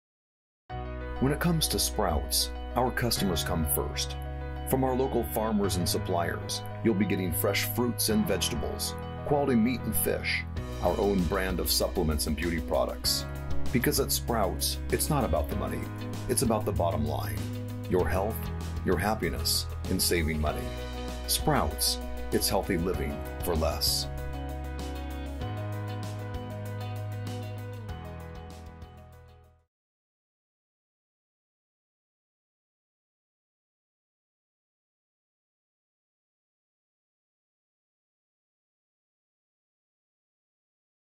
Sprouts Promo Commercial
This promotional video for Sprout's was created in Adobe Premiere Pro.  Animation was created in Adobe After Effects.  The vocals were recorded with the Zoom H4n.  Written and narrated by Andre Noe.